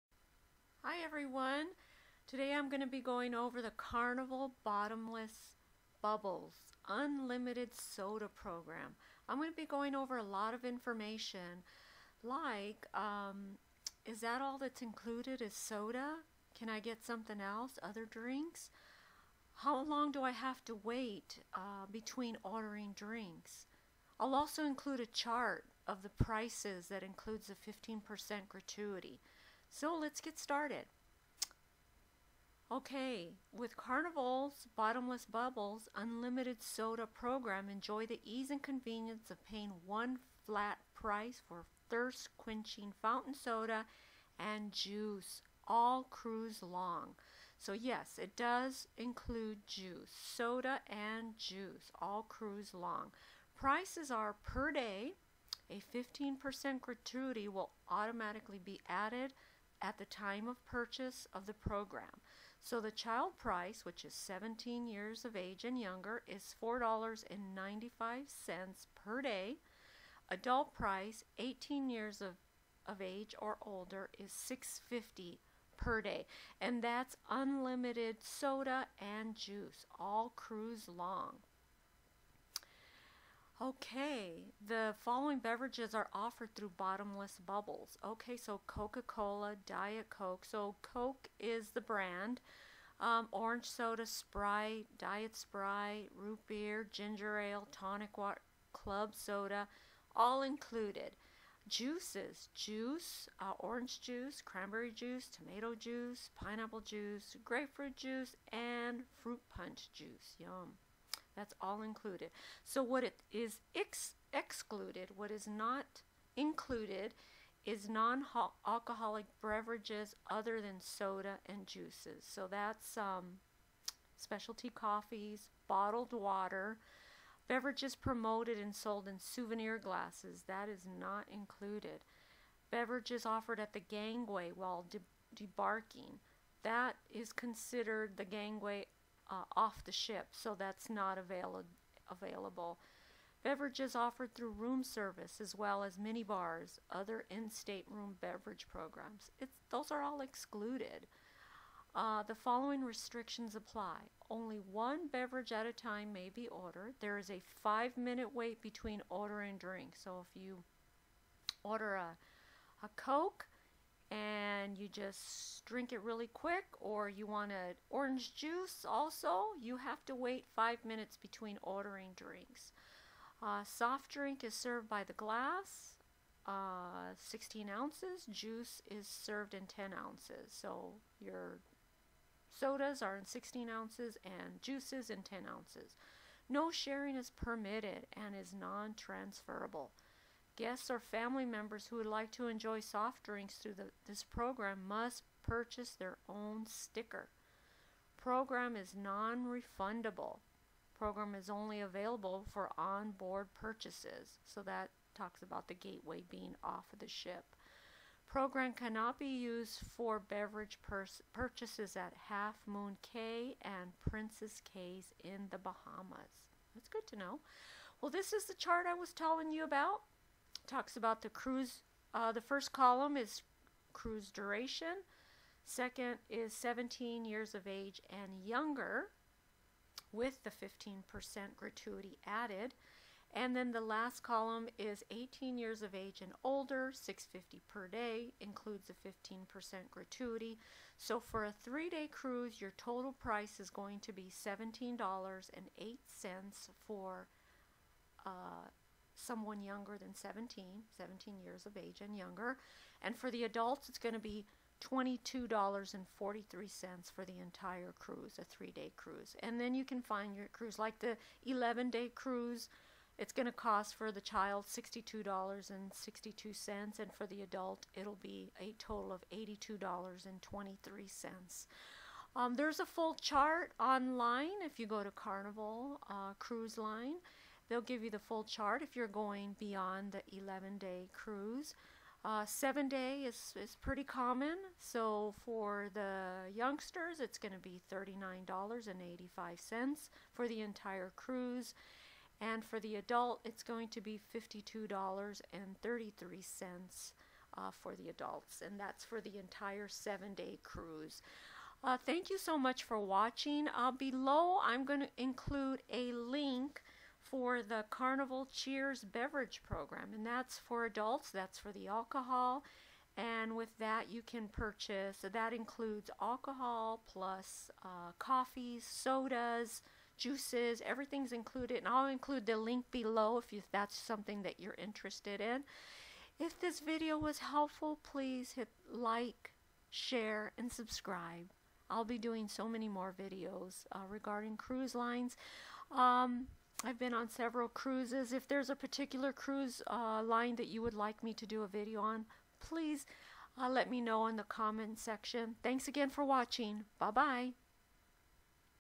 Carnival Cruise BOTTOMLESS BUBBLES Program 2017!
Sail refreshed with Carnival's Bottomless Bubbles unlimited soda program! Enjoy the ease and convenience of paying one flat price for thirst-quenching fountain soda and juice all cruise long!